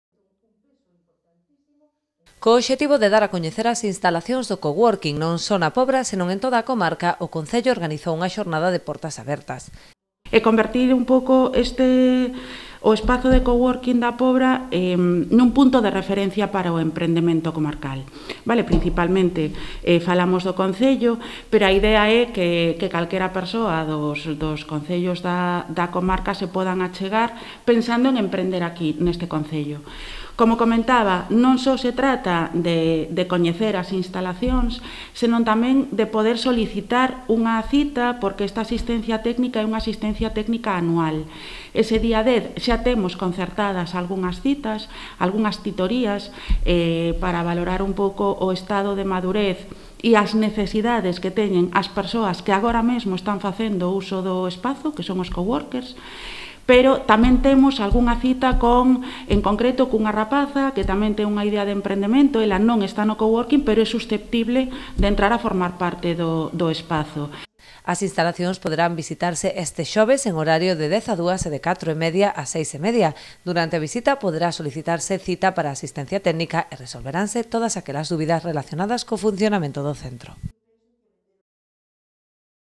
O Coworking da Pobra celebra este xoves unha xornada de portas abertas
O Coworking da Pobra acolle este xoves unha xornada de portas abertas. Aínda que está aberta á poboación en xeral, diríxese principalmente a novos emprendedores que poidan instalarse neste espazo colaborativo.